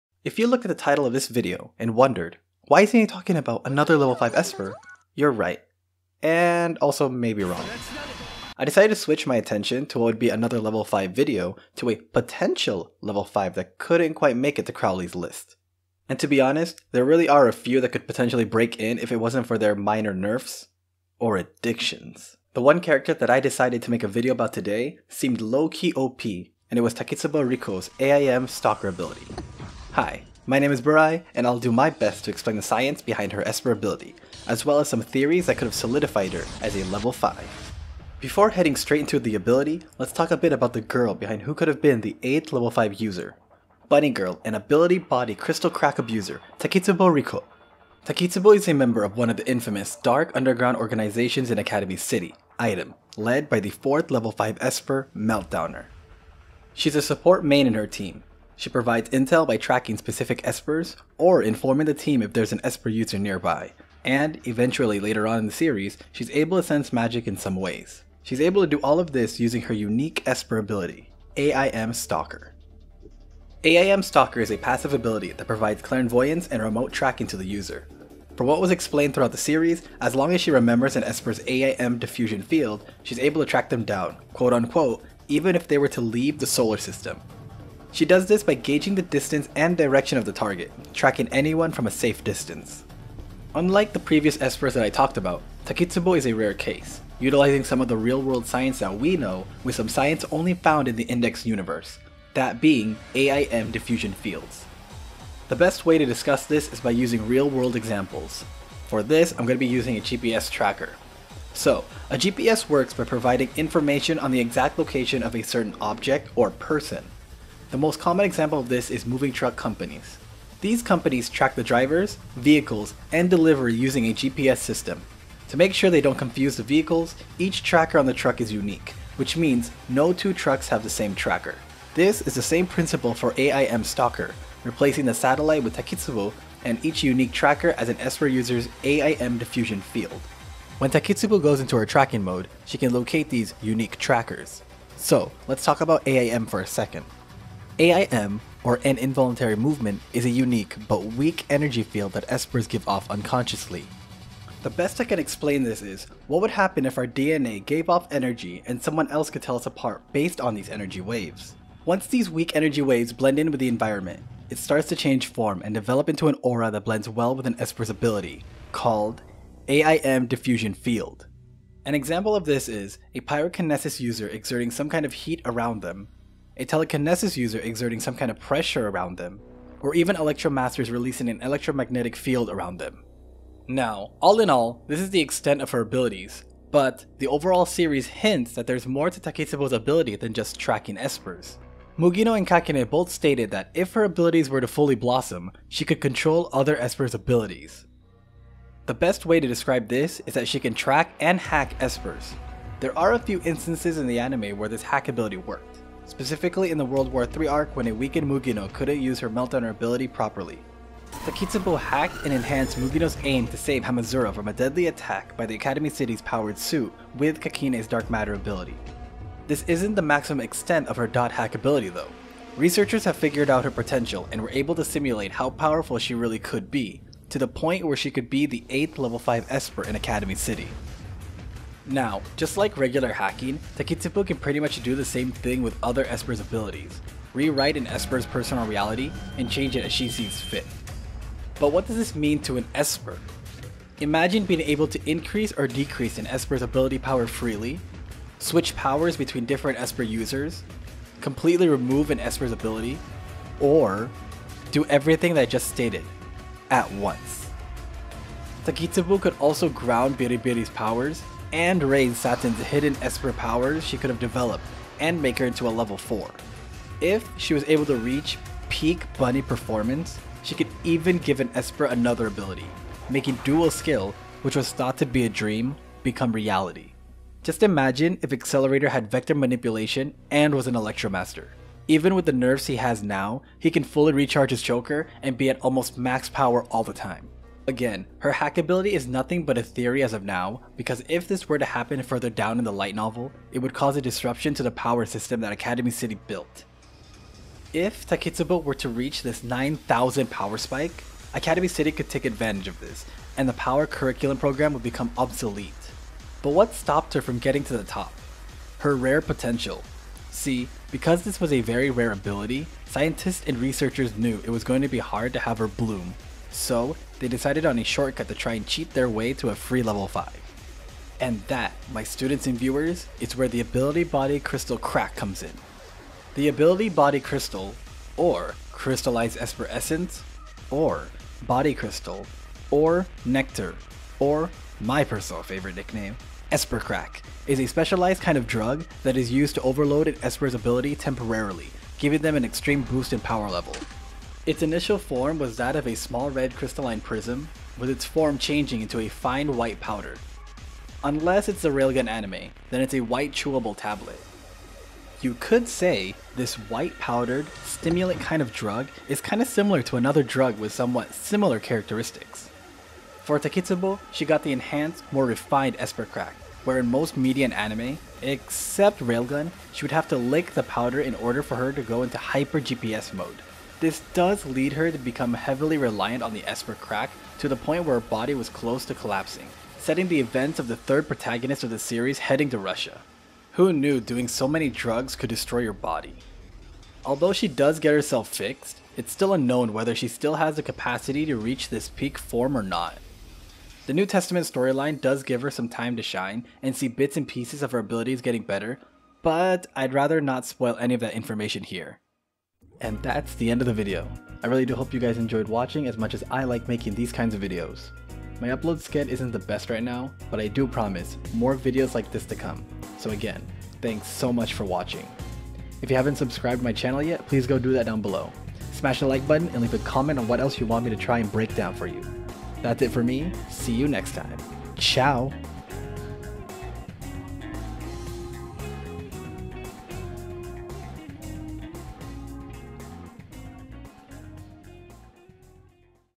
Science of Anime | AIM Stalker Explained
Stepping down a level to talk about a potential level 5 candidate, Takitsubo Rikou from the To Aru series. Explaining how her esper powers work and some extra explanation on her potential level 5 ranked 8 esper powers.

This video took way too long cause of IRL stuff, but I hope you all enjoy the video.

Anime Series: To Aru Majutsu no Index / To Aru Kagaku no Railgun / To Aru Kagaku no Accelerator (A Certain Magical Index / A Certain Scientific Railgun / A Certain Scientific Accelerator)